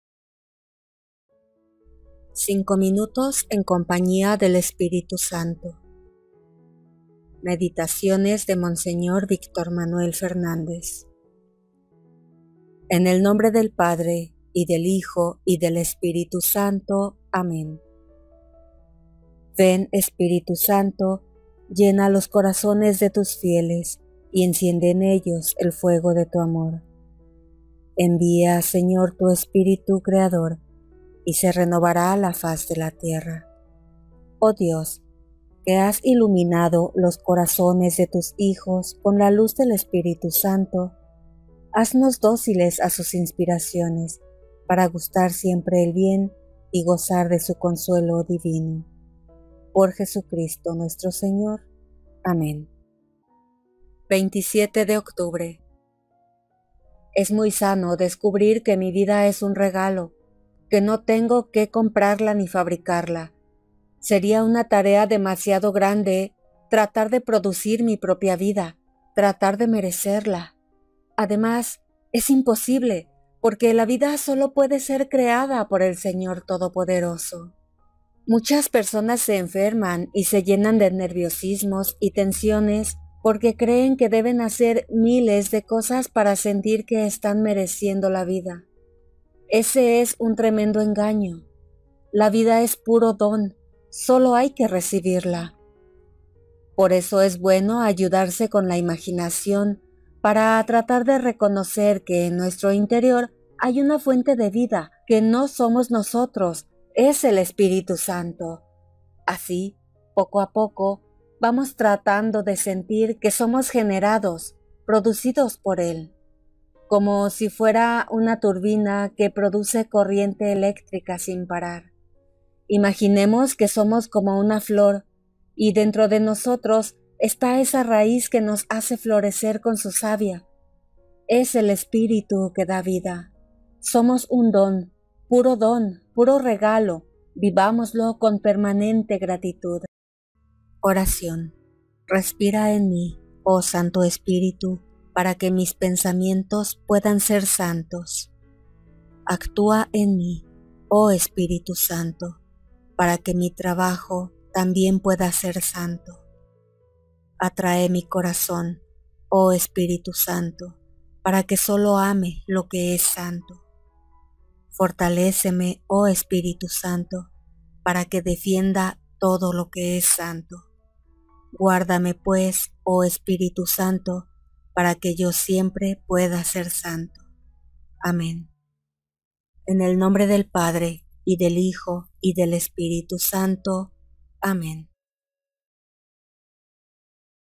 los 5 minutos con el Espíritu Santo 27 de octubre
En estas meditaciones y oraciones el padre Víctor Manuel Fernández nos ofrece la posibilidad de recorrer a lo largo del año el sublime camino interior de la mano del Espíritu Santo. Publicaciones Claretianas

Compra el libro en amazon

los cinco 5 minutos del con el Espíritu Santo

27 de Octubre

Es muy sano descubrir que mi vida es un regalo, que no tengo que comprarla ni fabricarla. Sería una tarea demasiado grande tratar de producir mi propia vida, tratar de merecerla. Además, es imposible, porque la vida sólo puede ser creada por el Señor todopoderoso.
Muchas personas se enferman y se llenan de nerviosismos y tensiones porque creen que deben hacer miles de cosas para sentir que están mereciendo la vida. Ese es un tremendo engaño. La vida es puro don, sólo hay que recibirla.
Por eso es bueno ayudarse con la imaginación, para tratar de reconocer que en nuestro interior hay una fuente de la vida, que no somos nosotros, es el Espíritu Santo. Así, poco a poco, vamos tratando de sentir que somos generados, producidos por él, como si fuera una turbina que produce corriente eléctrica sin parar.
Imaginemos que somos como una flor, y dentro de nosotros está esa raíz que nos hace florecer con su sabia. Es el Espíritu que da vida.
Somos un don, puro don, puro regalo. Vivámoslo con permanente gratitud.